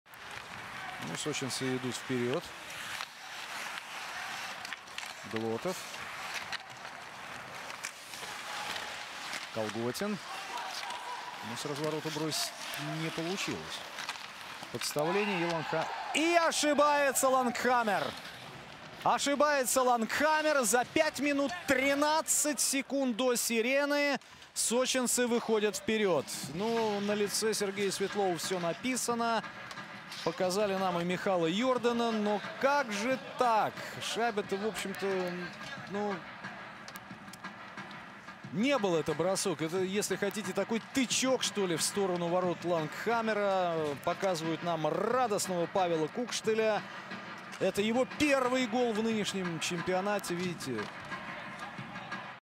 Kukshtel first KHL goal
Pavel Kukshtel scores his first KHL goal

Павел Кукштель забрасывает первую шайбу в КХЛ, которая стала и победной в матче